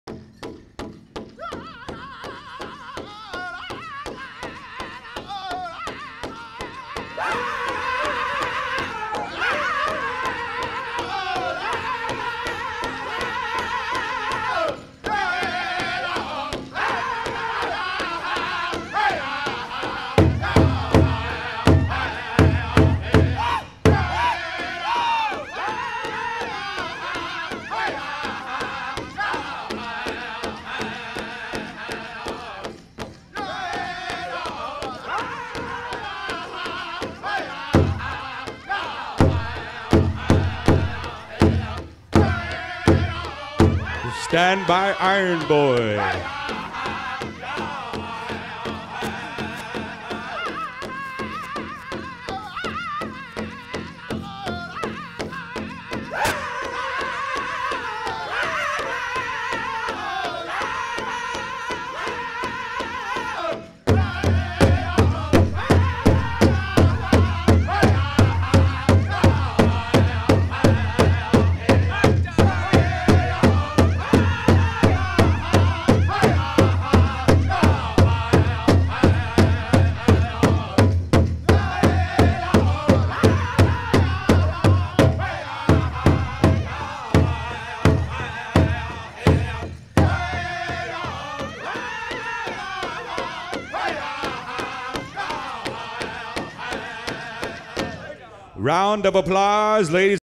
Battle Royale - Men's Grass Pt 9 - 2018 United Tribes Pow Wow - PowWows.com
Battle Royale - Men's Grass Part 9
2018 United Tribes Pow Wow
Bismark, ND